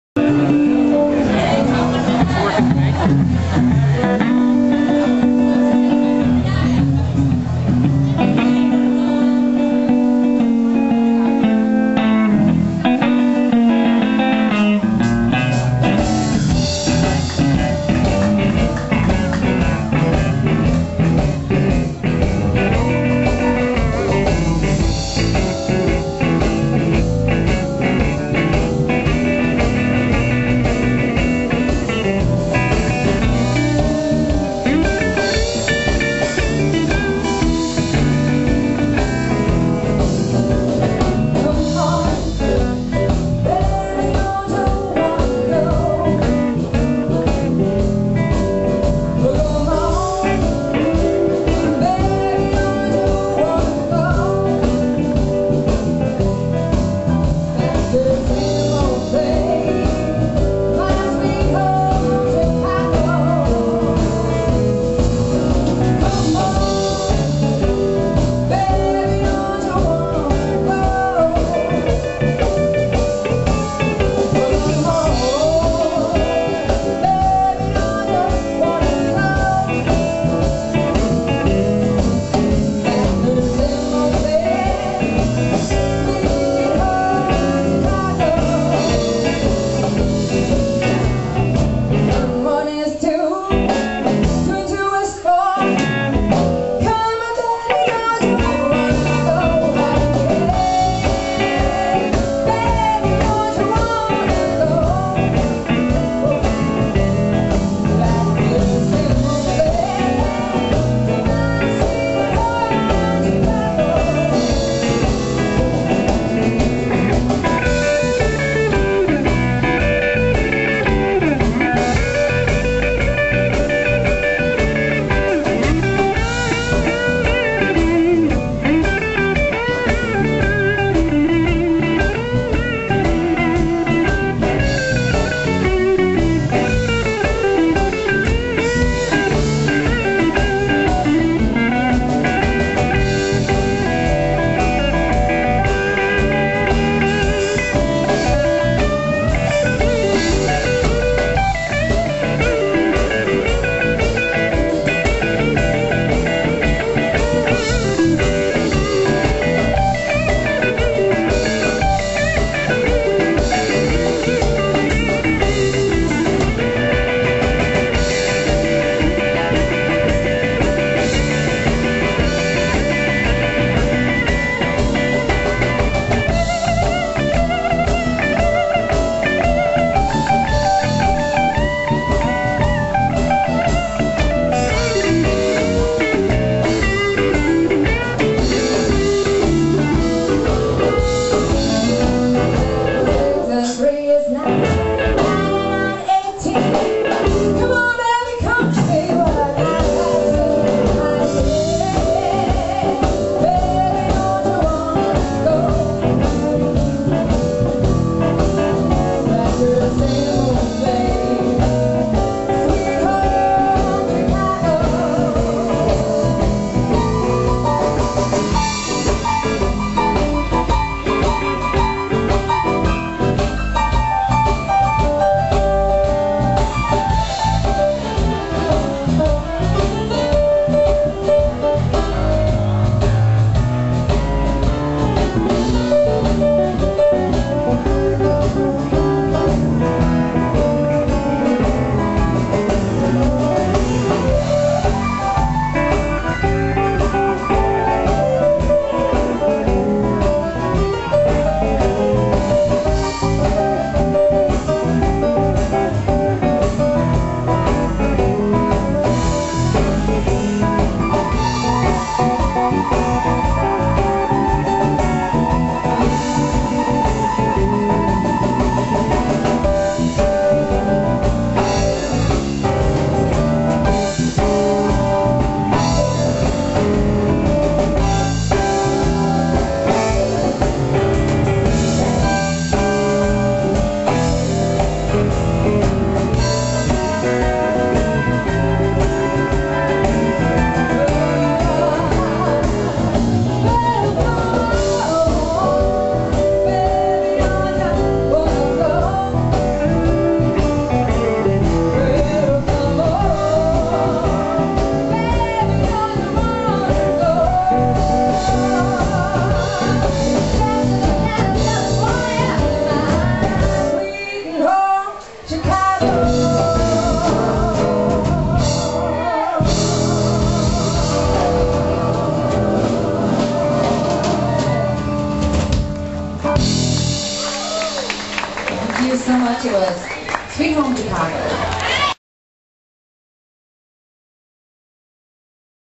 Peds Band Nov26#1
Ped's Blues Band play Friday and Saturday nights at the great live music bar, Saxophone Pub, near Bangkok's Victory Monument. One of the stars of a fast-growing blues scene in Thailand, Ped is a master guitarist who could get a job anywhere in the world; his band are hot too.